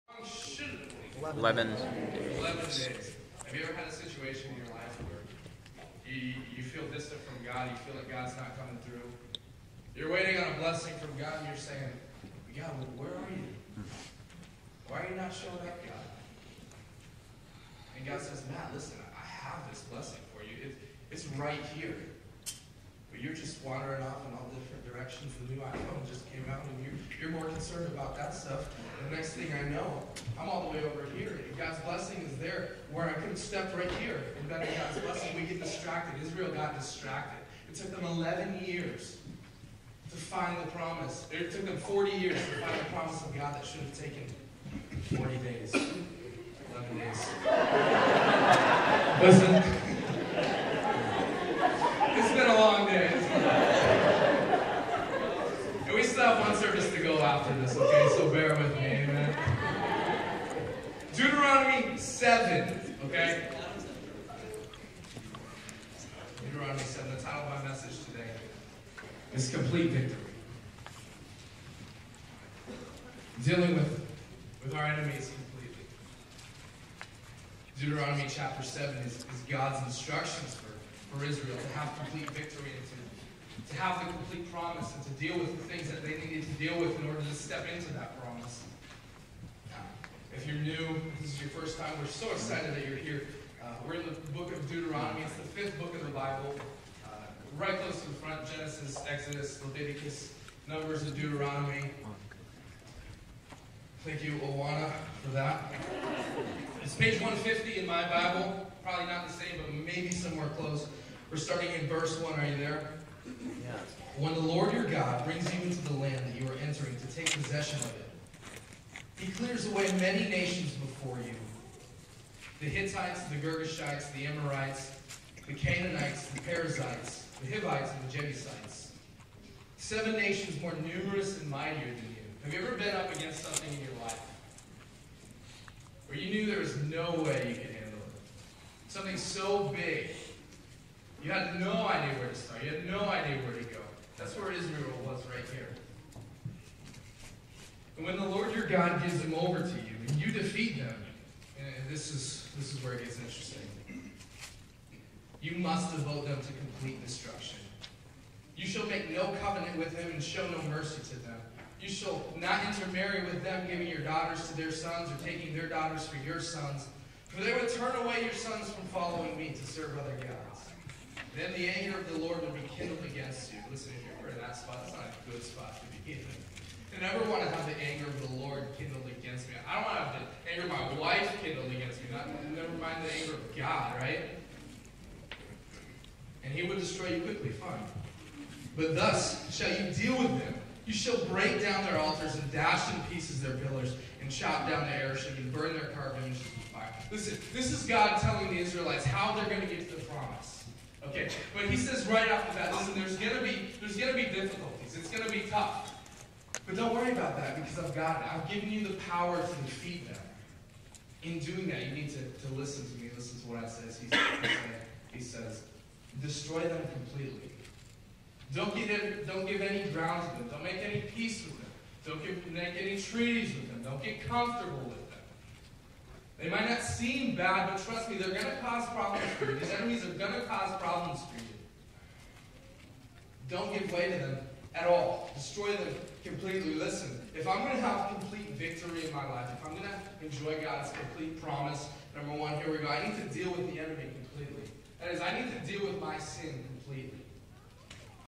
Matt Whitacre Preaching this Morning at Life Point Church
Life Point Church
January 24, 2016 service
Life Point Church, Chicopee, MA